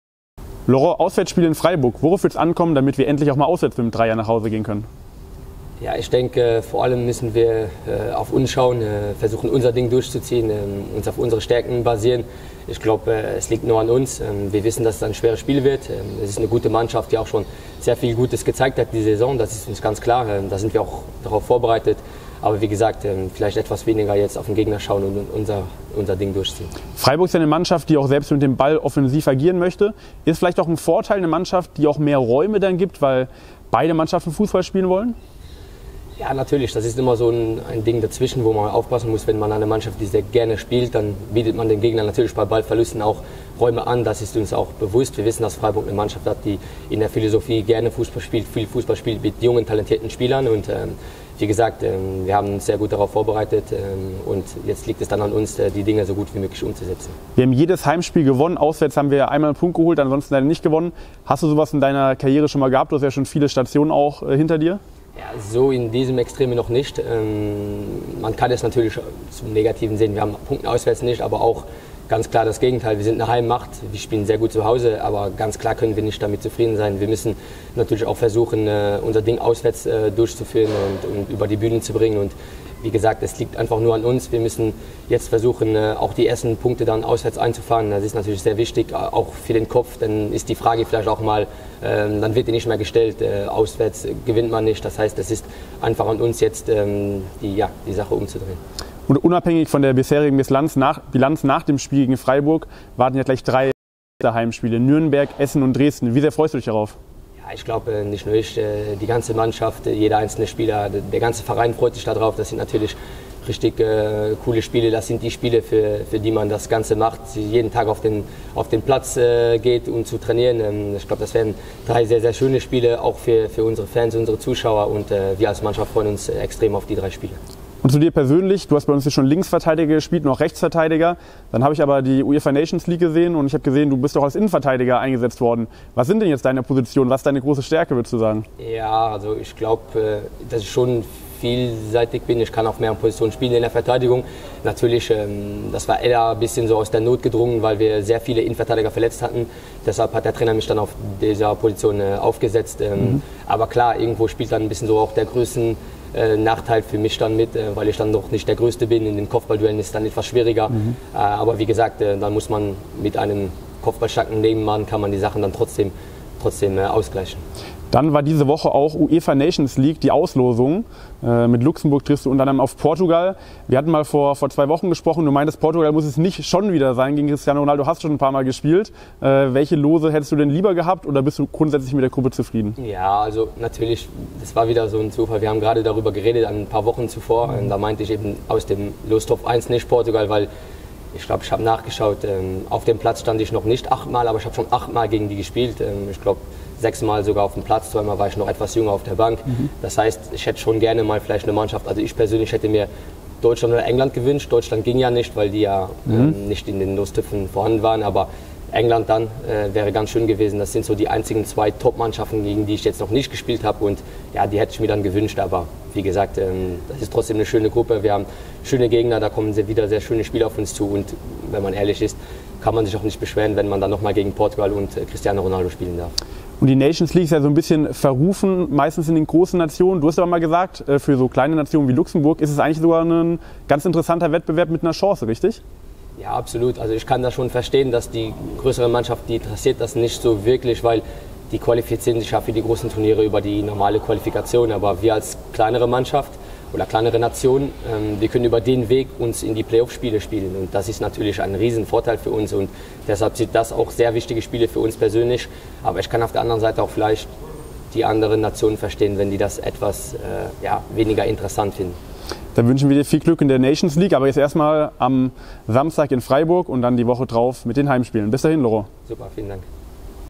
3. Liga | 12. Spieltag | SC Freiburg II - SV Waldhof | AUUUFWÄRMEN mit Laurent Jans
Vor dem Auswärtsspiel am kommenden Samstag gegen den SC Freiburg II haben wir uns in unserem Format "AUUUFWÄRMEN" mit Laurent Jans über die anstehende Aufgabe unterhalten.